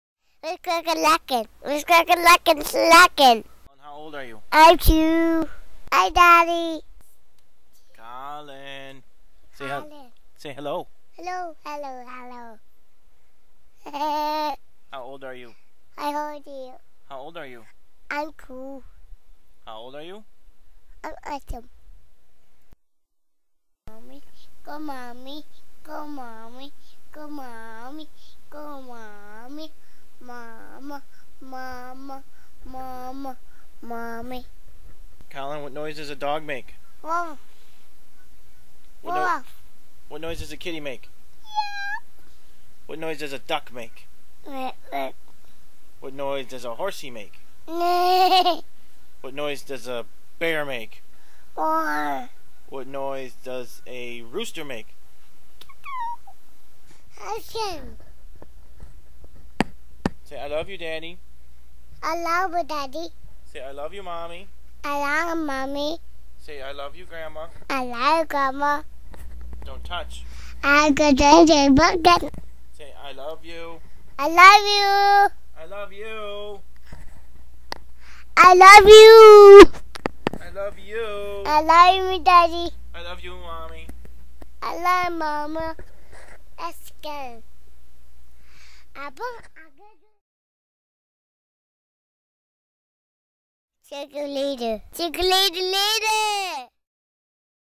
Colin Video
I found some old audio files and put this together.  I only have windows movie maker on my pc so the editing is rather poor.